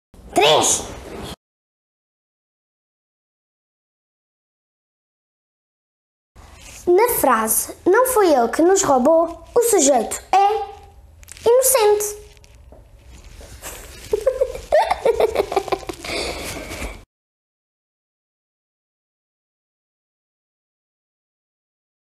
0070 Não foi ele que nos roubou | Carolina Anjo
As adivinhas fazem parte da literatura popular antiga que consiste na interrogação engenhosa que convida à decifração. Começamos com uma pergunta e geralmente a resposta é engraçada. Nas adivinhas é usada a lógica e os trocadilhos o que as torna atrativas aos mais jovens.